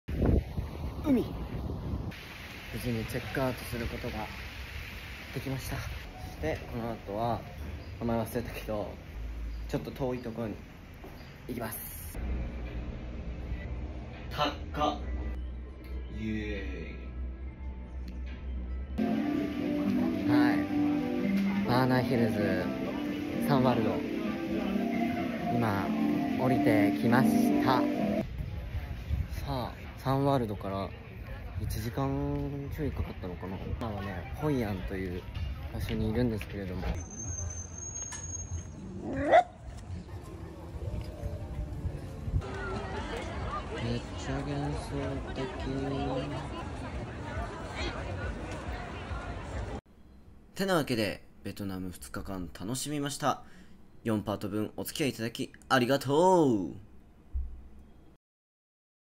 美 少年【藤井直樹ベトナムVlog④】ドキドキワクワクの一人旅〜撮影編集全て藤井直樹〜
藤井直樹（美 少年)のベトナム一人旅Vlogです！
自分で撮影して編集しました！